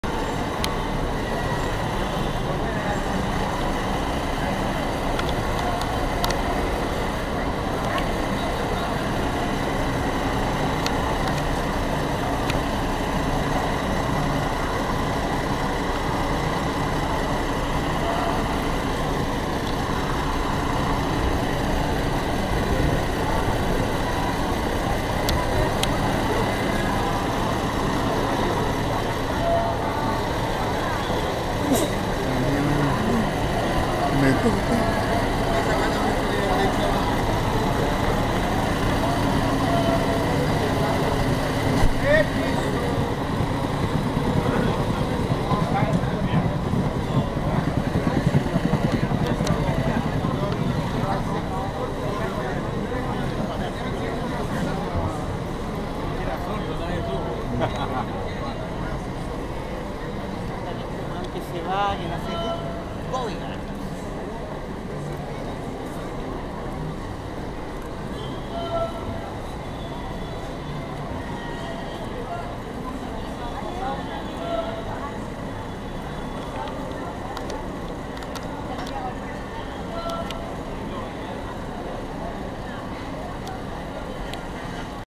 Camión de Bomberos Renault Camiva verde en Santiago Chile
Con las sirenas parpadeando encontré este carro de bomberos en el Paseo Ahumada de la 5° Compañia de Bomberos Arturo Prat de Santiago